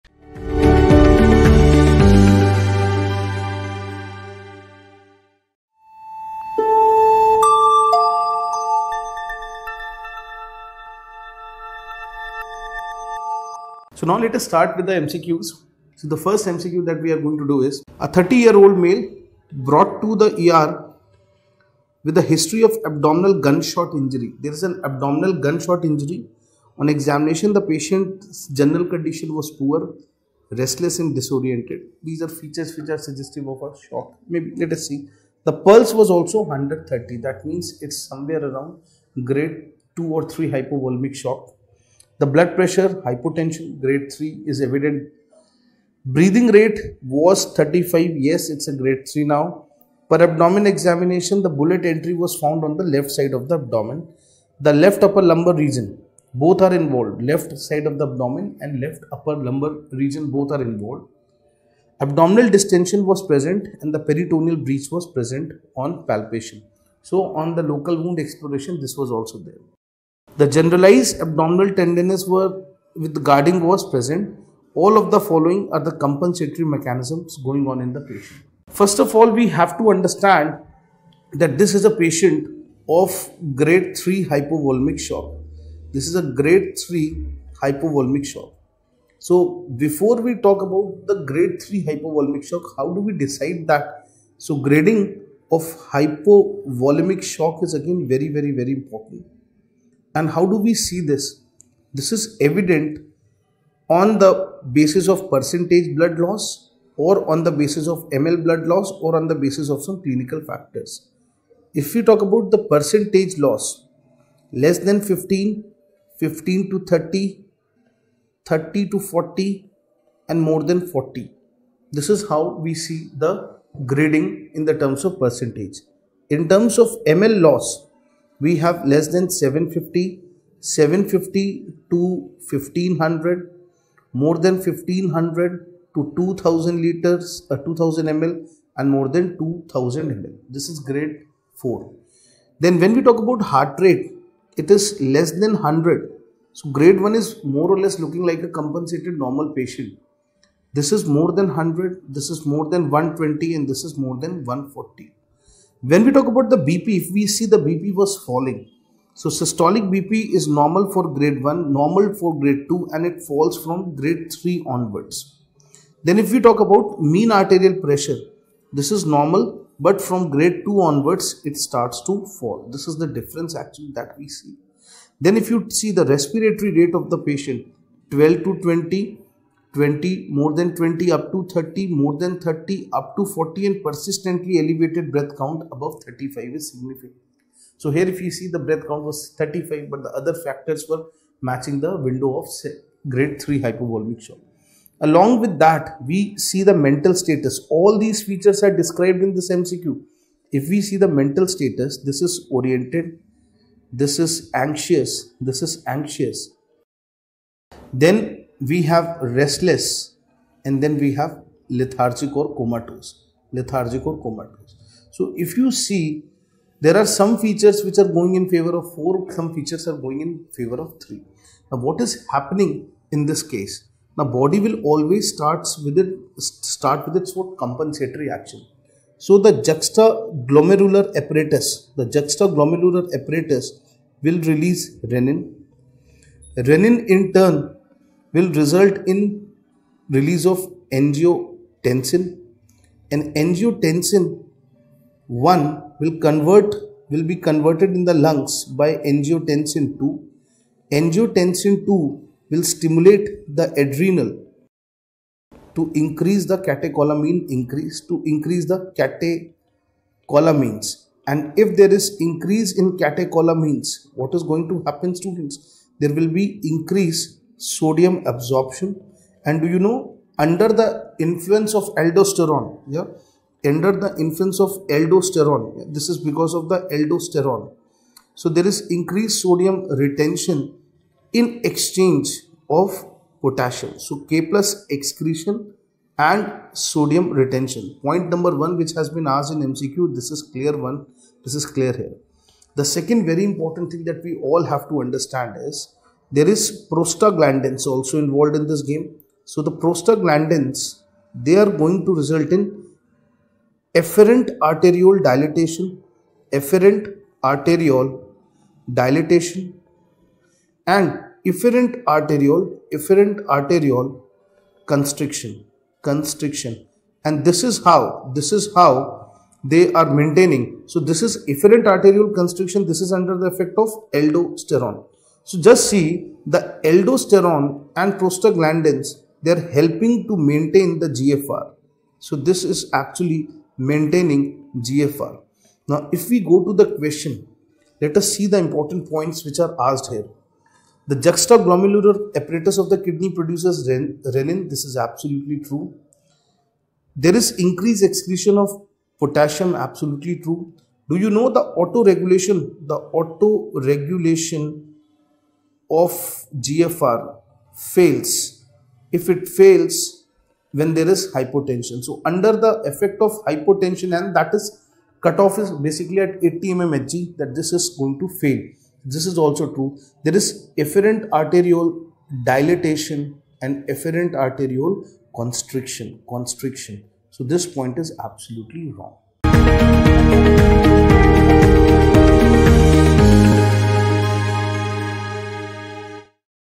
Abdominal Gunshot Injury [General Surgery] - NEET SS MCQ Discussion
Watch MCQ Based Discussion: Abdominal Gunshot Injury, by PrepLadder’s NEET SS Faculty for General Surgery - Dr. Saurabh Dixit. Watch this video to clear all your doubts.